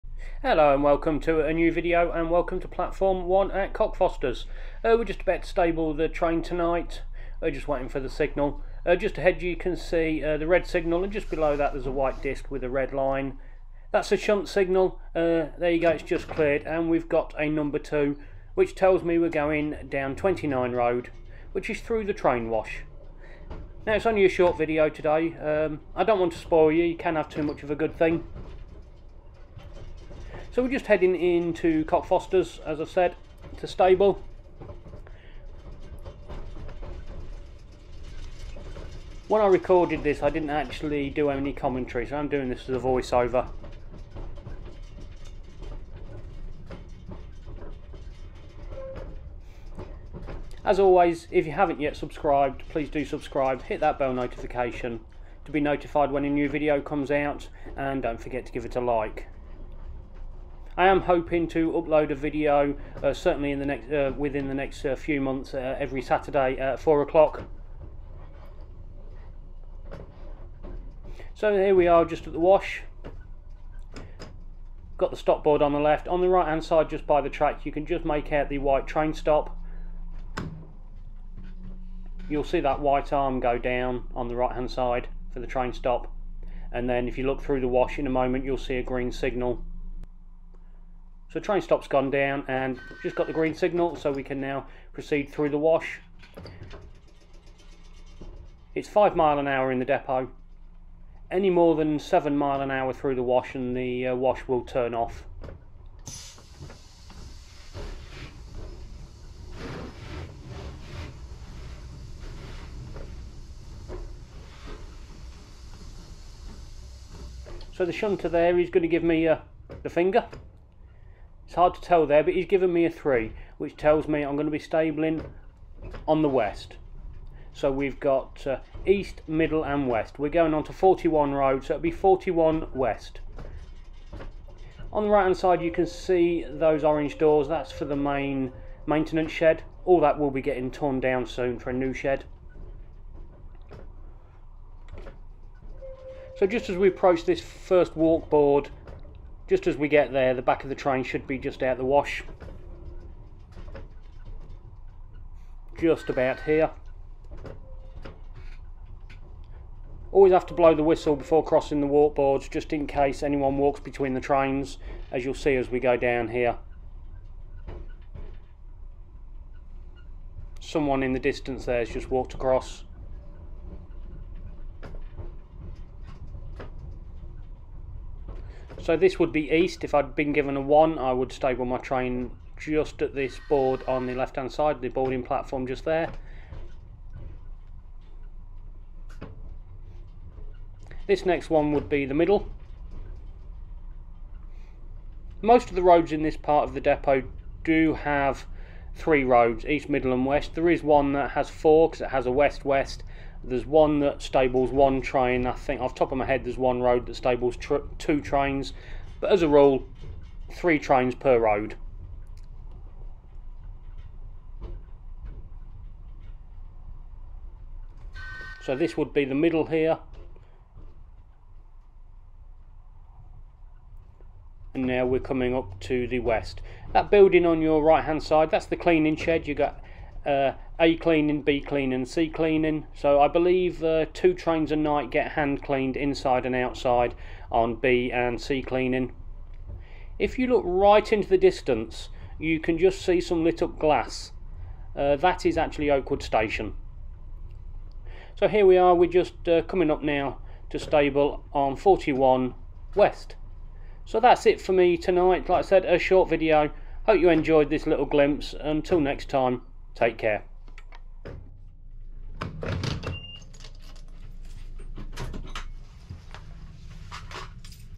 Ever wanted to know how we wash the trains? Stabling the train at Cockfosters. Piccadilly line
Hello and welcome to a new video.
This weeks video is a short one.

In tis video I stable the train from Cockfosters platform to Cockfosters depot via the train wash.
So if you have ever wanted to know how we wash the trains now is your chance.